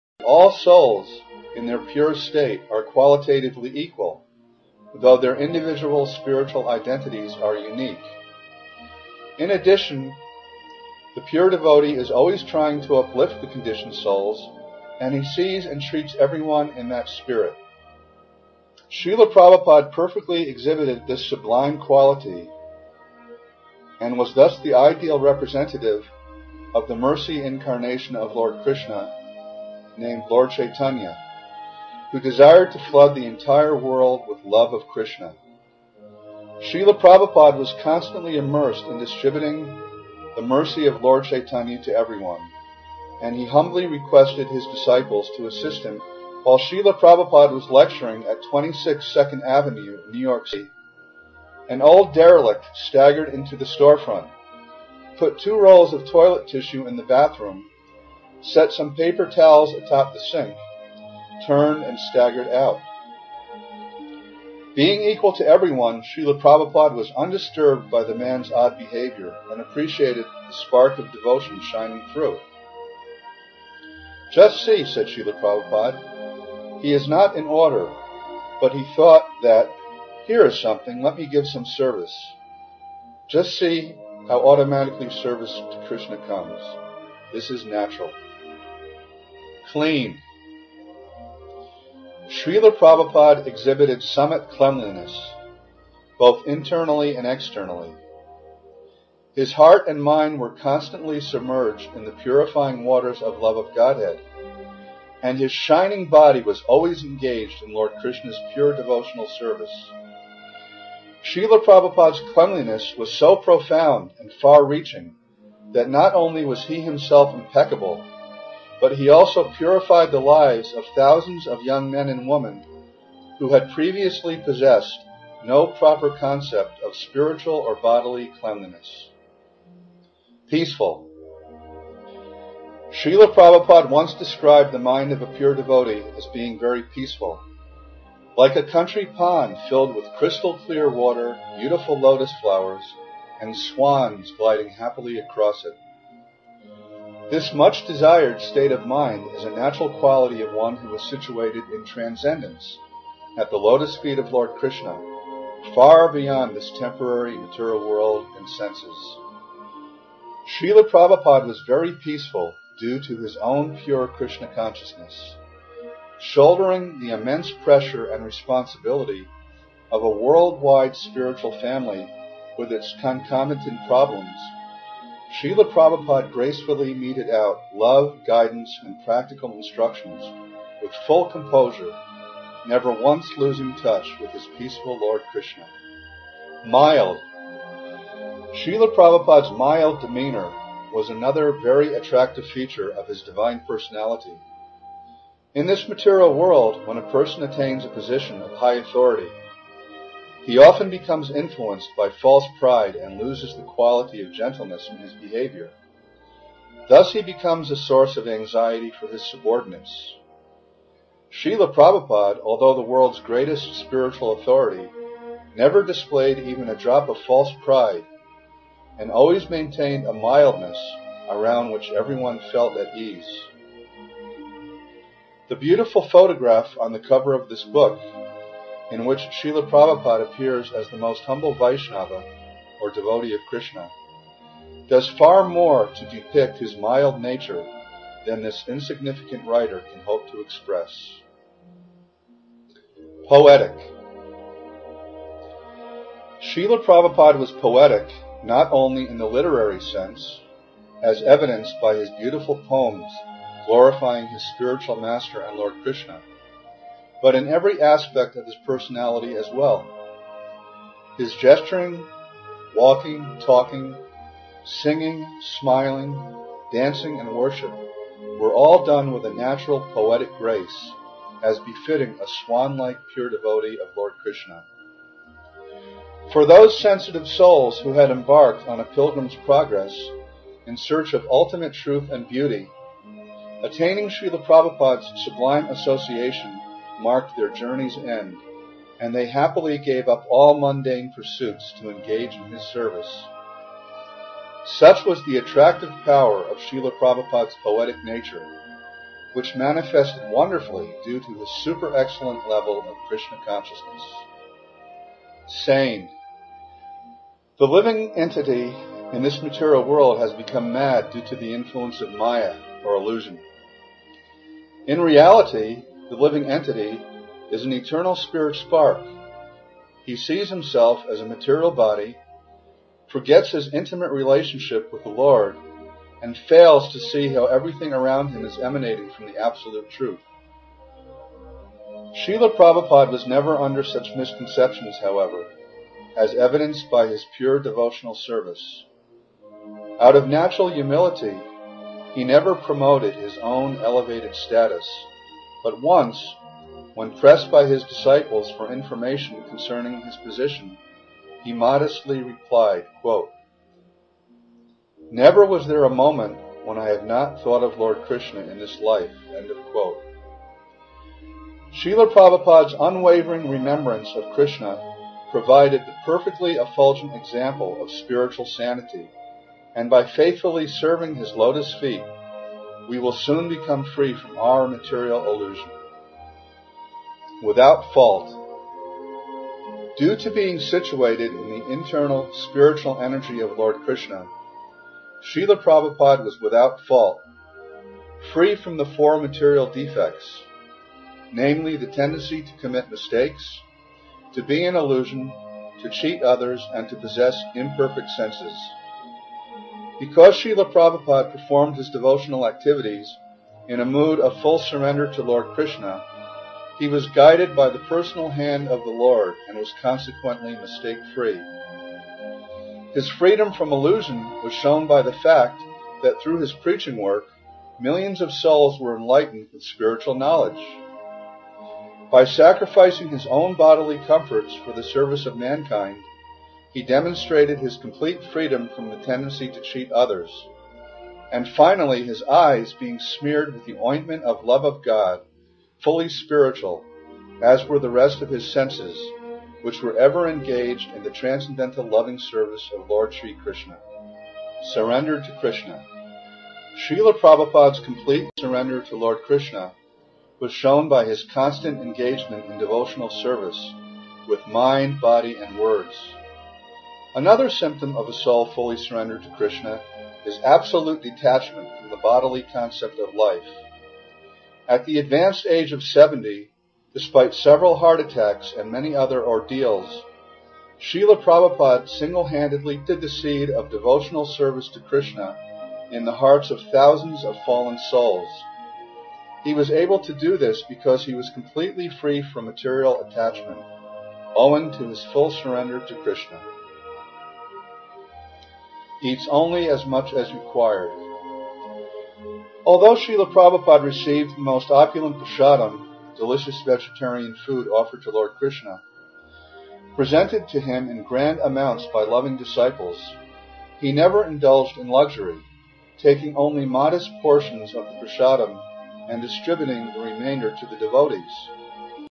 Srila Prabhupadas Beautiful Transcendental Qualities (3 of 4)
The Nectar of Devotion offers an illuminating summary of Lord Krishnas spiritual qualities. By studying this great work in a spirit of devotion, we can enhance our appreciation for the Lord and thus develop the desire to know His sublime friendship. For example, the Nectar of Devotion explains that no one is more appreciative than Lord Krishna, as shown in His dealings with His friends. Once, a poor devotee named Sudama offered Krishna a few grains of rice. Because he was penniless, he was unable to present the Lord with a valuable gift, as was his desire, but because his humble offering was saturated with love, Krishna eagerly accepted it and ate it with great delight. Out of deep gratitude, Krishna reciprocated with Sudama by giving him more opulence than can be imagined even by the wealthiest person in this world, and in the end he was granted entrance into Krishnas spiritual abode.